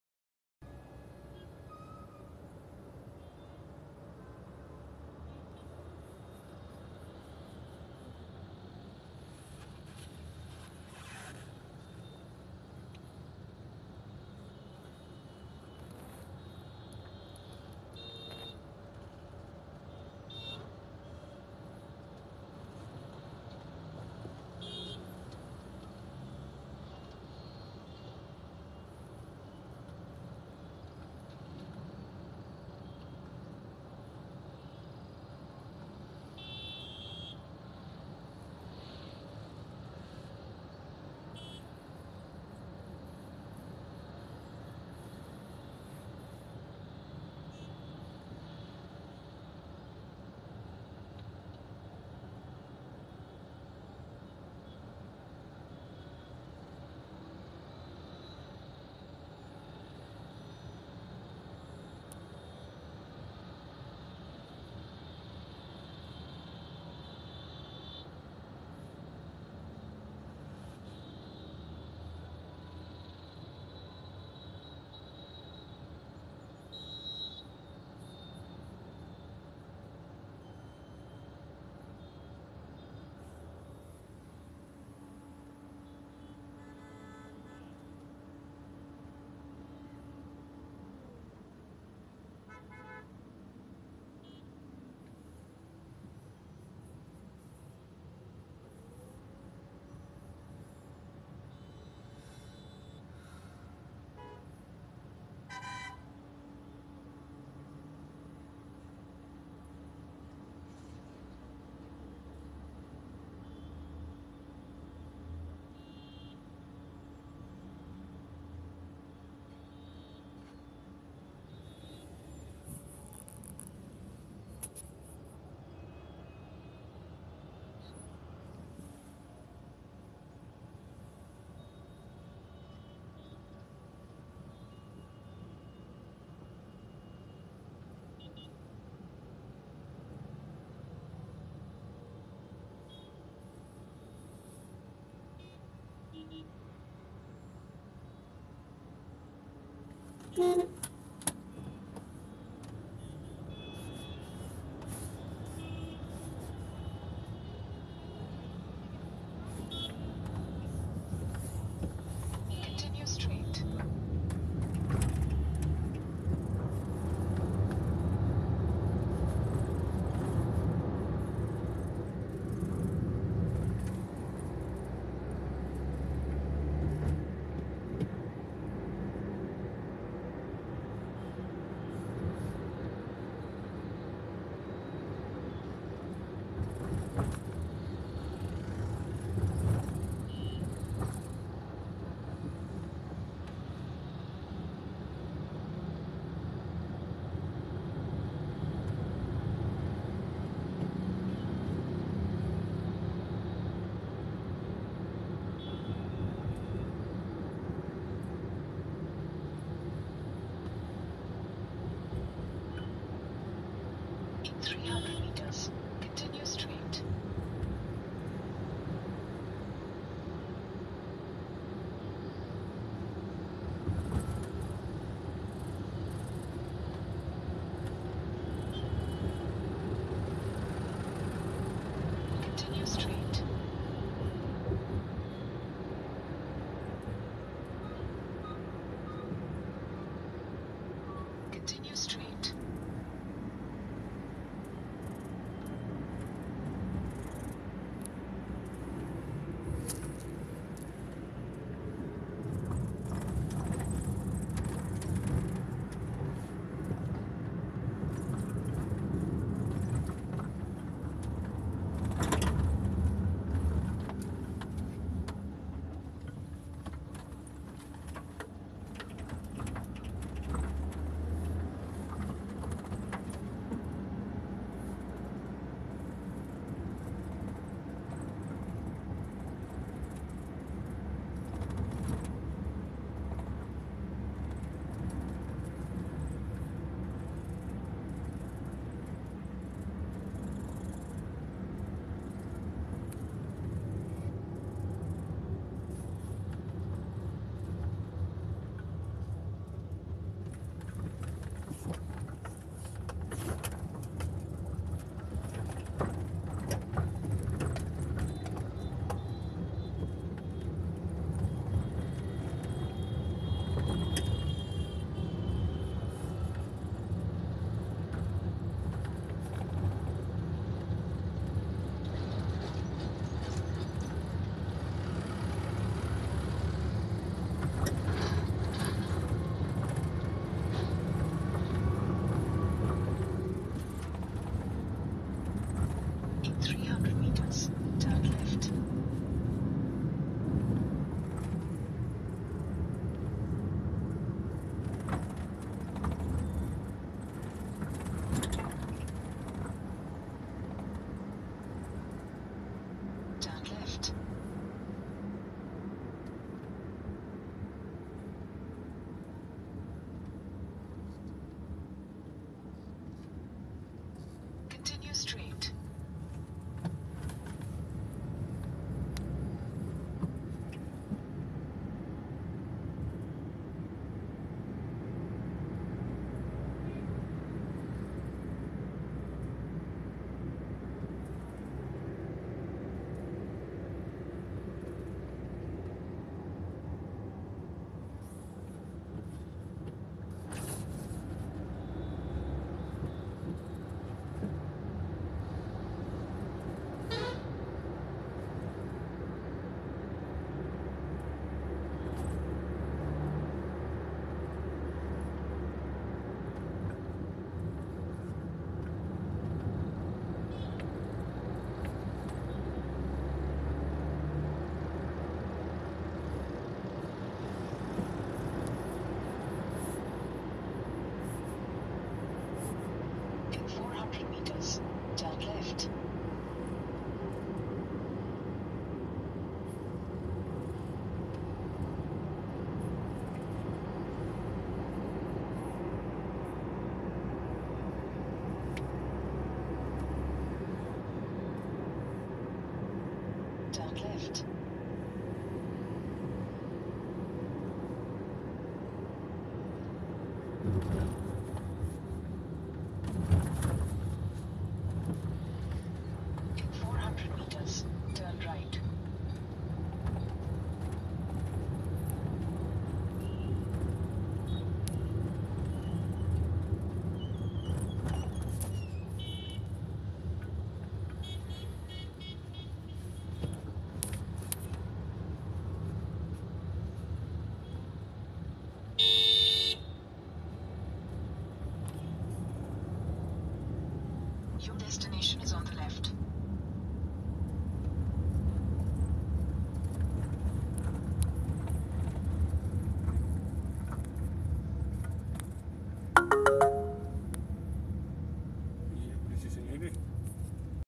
THEY SAY I DRIVE LIKE BHAGAT SINGH | MOXAMOVIE | BY RODDUR ROY
A Postmodernist One Shot Movie on Friving n the Bhagat Singh's Realization n appreciation of his vision in Symbolic City Scape in MoxaMovie Style of Making!